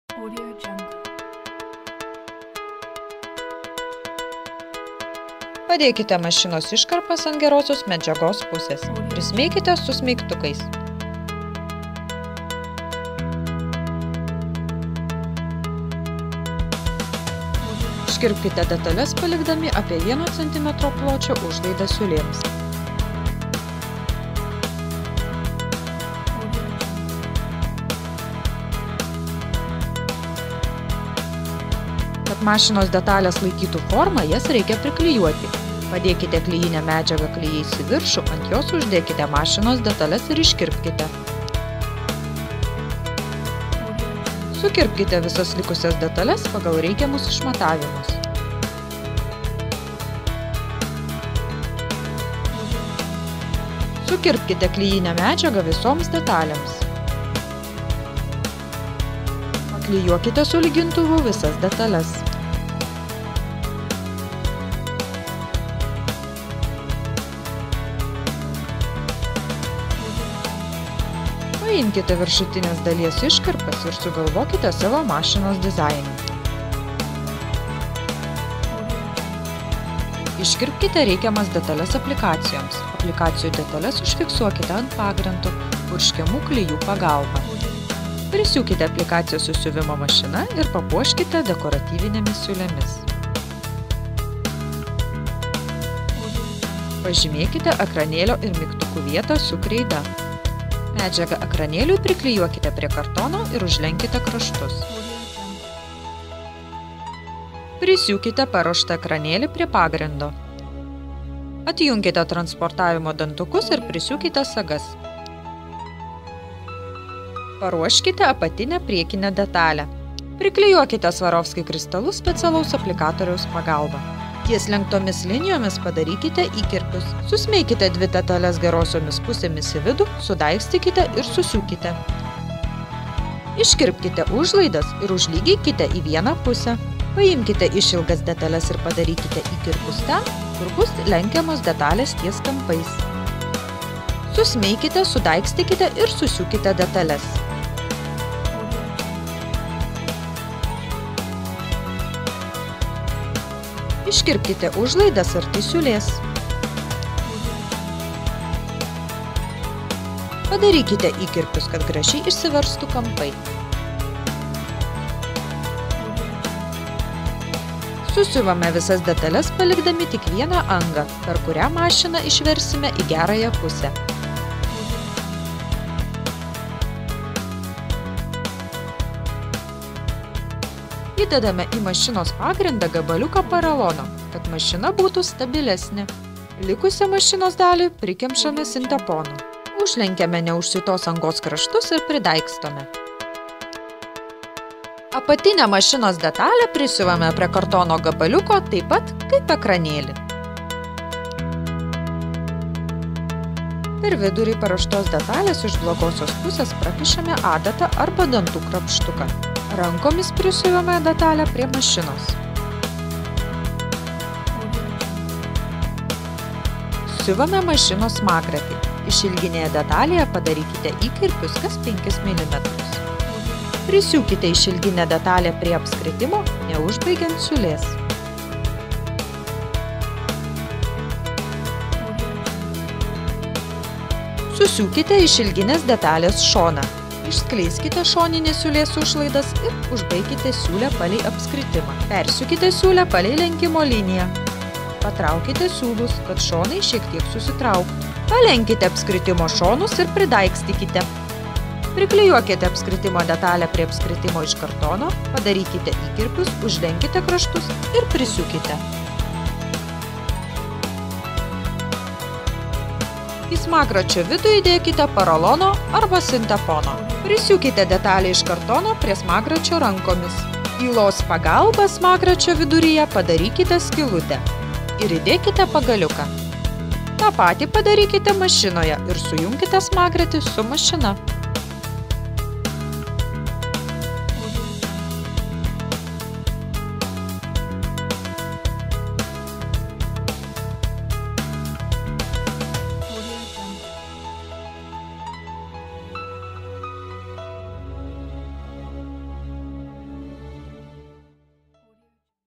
Siuvame Bernina
Siuvame siuvimo mašinos Bernina modeliuką.